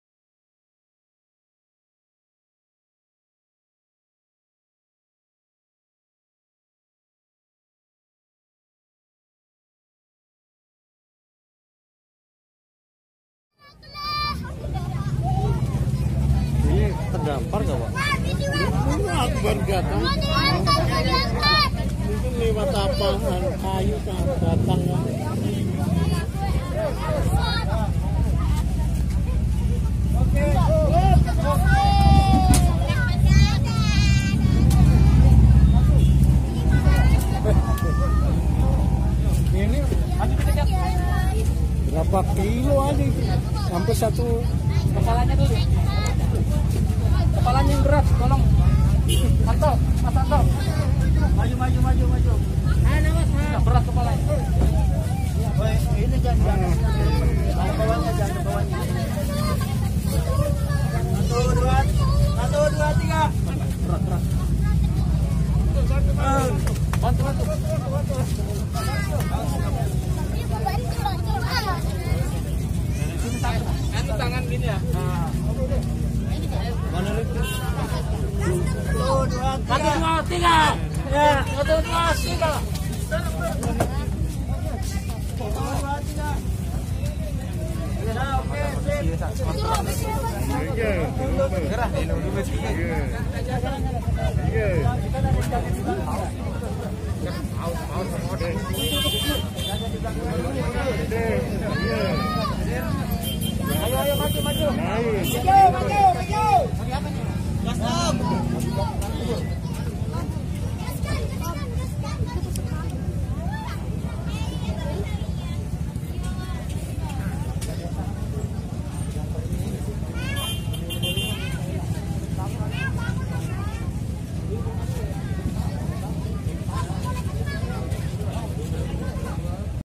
Lumba-lumba mati Terdampar, warga sampai heboh
Se ekor lumba2 d temukan terdampar d sungai segah kabupaten berau kalimantan Timur, na'as sekali kondisi dalam keadaan sudah tewas.. Tim setempat beserta warga membantu proses evakuasi